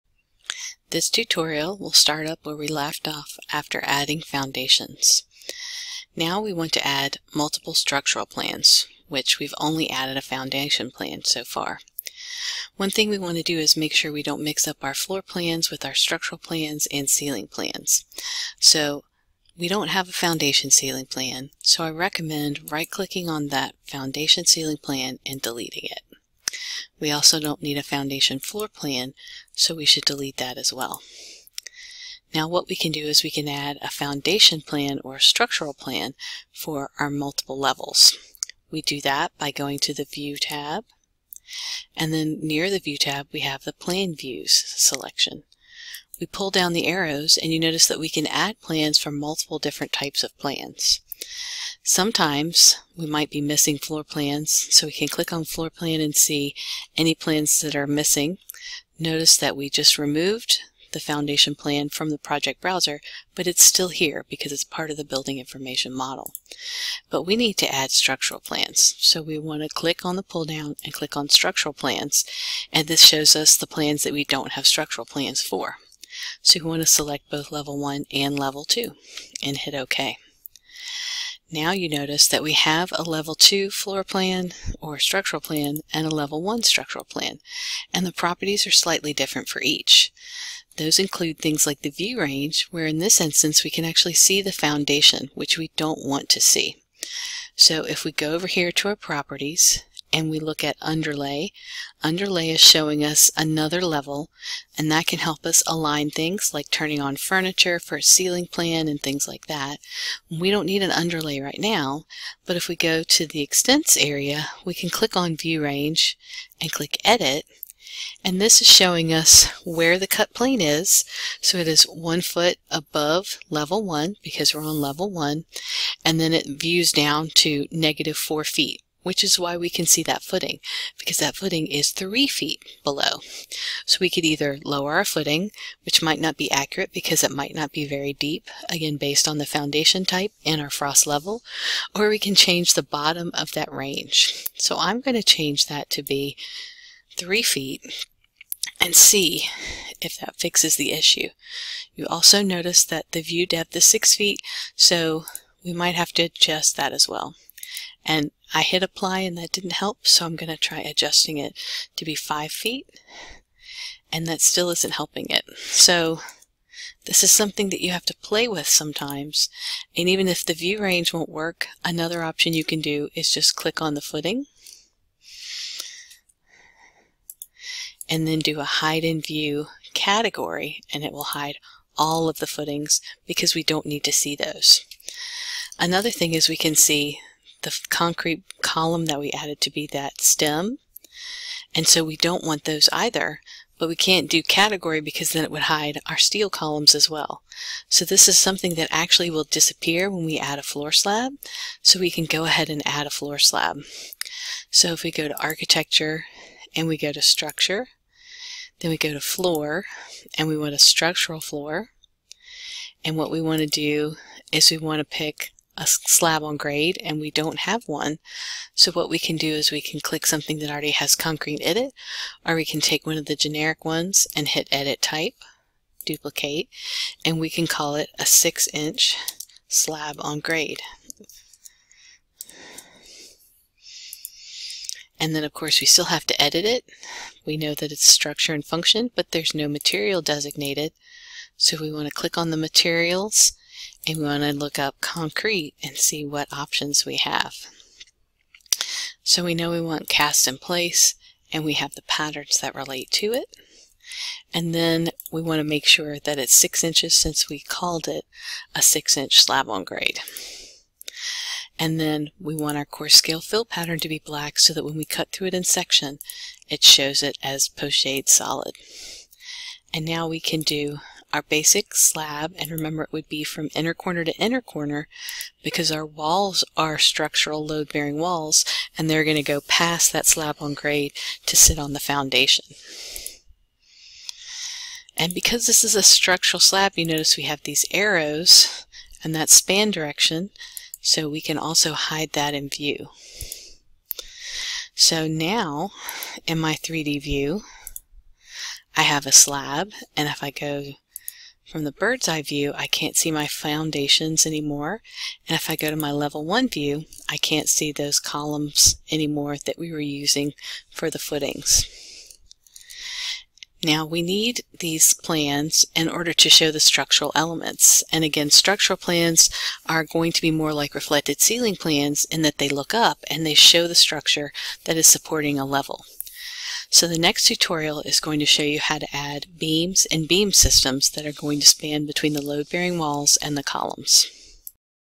Adding Structural Plans
Revit tutorial by Mississippi State University School of Architecture Associate Professor Alexis Gregory